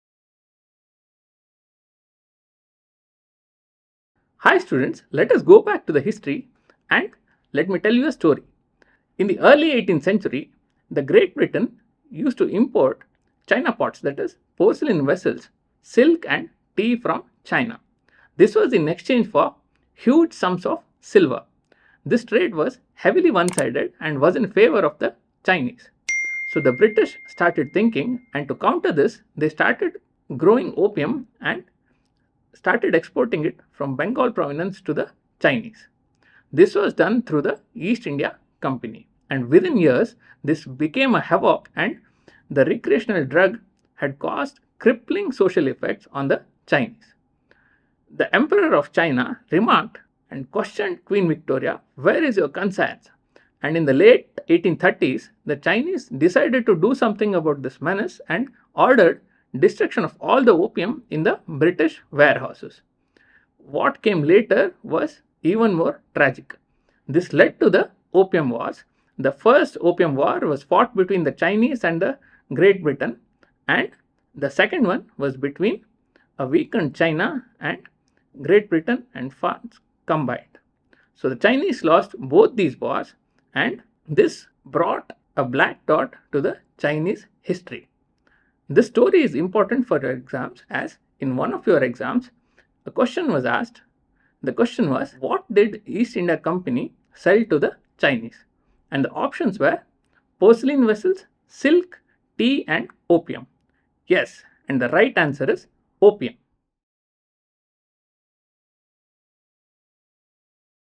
NEET-PG Prep | Anesthesia| The Opium Wars by Dr Ganapathy Hegade
Attention NEET-PG Aspirants! With the Opium Wars, Dr. Ganapathy Hegade provides critical insights that can't be missed. As NEET-PG 2024 draws near, it's time to accelerate your revision. Join us now to ensure you're fully equipped to tackle the subject - Anesthesia and ace the exam.